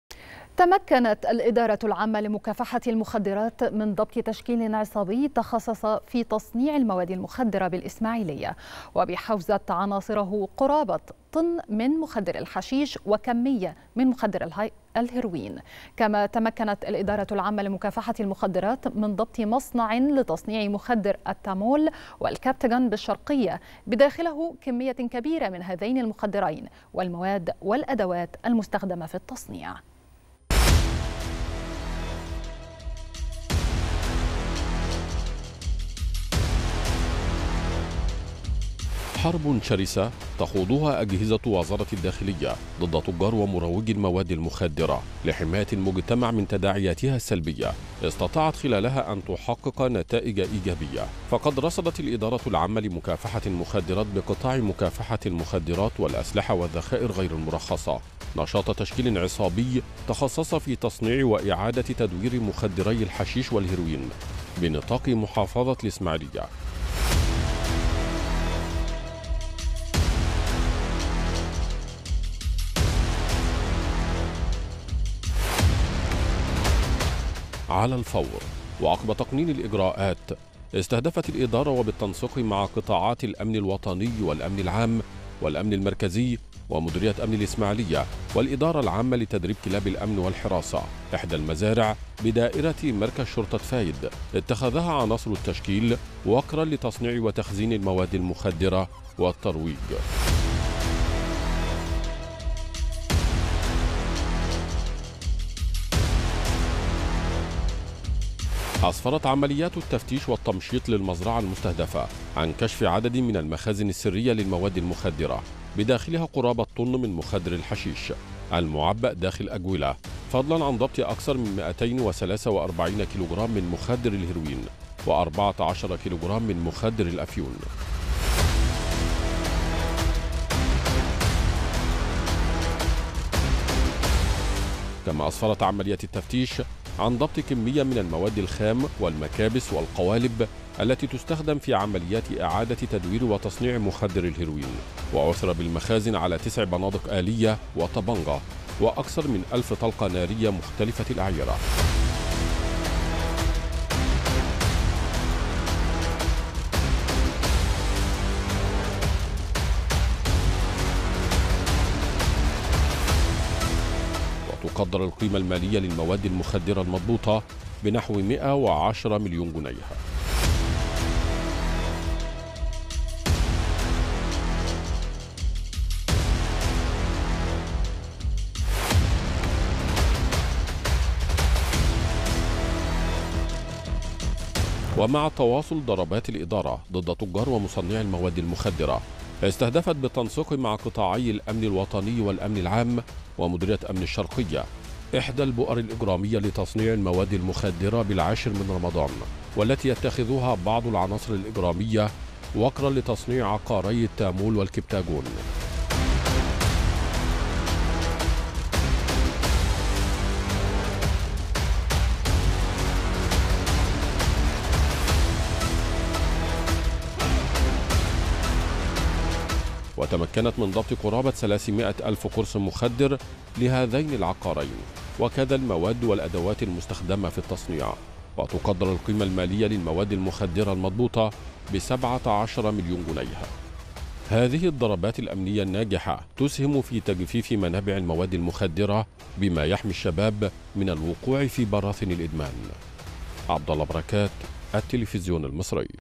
الإدارة العامة لمكافحة المخدرات تتمكن من ضبط تشكيل عصابي تخصص في تصنيع المواد المخدرة بالإسماعيلية
نشرة السادسة | الإدارة العامة لمكافحة المخدرات تتمكن من ضبط تشكيل عصابي تخصص في تصنيع المواد المخدرة بالإسماعيلية